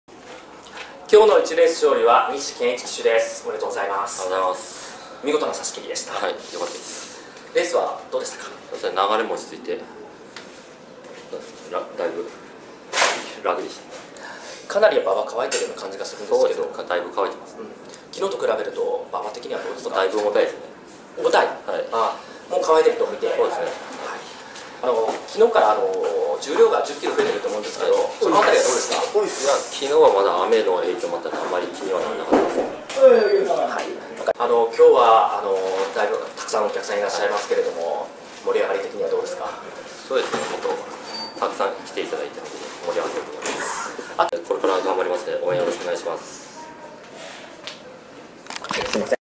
西謙一騎手2019年10月6日(日)～ばんえい競馬～
秋恒例のお祭りとかちばん馬まつり開催中。今日は人間ばん馬も行われ盛り上がりを見せています。天候にも恵まれ行楽日和です。